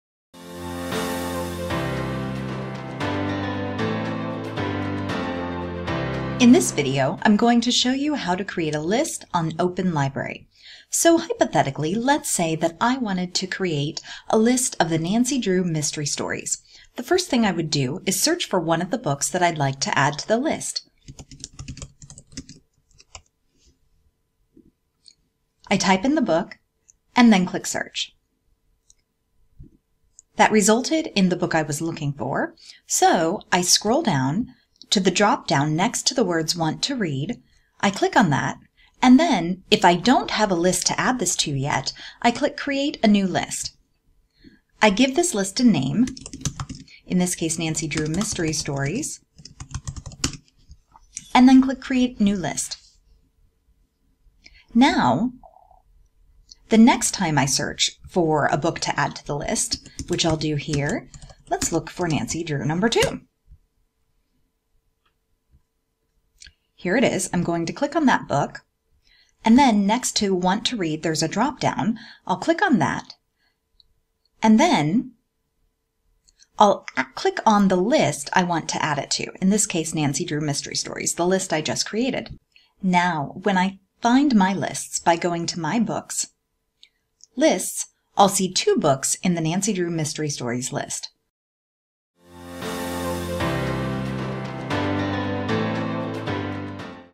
How to Create and Add to a List in Open Library
This video will show you how to create and add to a list in Open Library.

Open Library can be a great tool for creating reading lists, either for your students, or your own scholarship and research.

Full disclosure: I volunteer for Open Library and created this as documentation.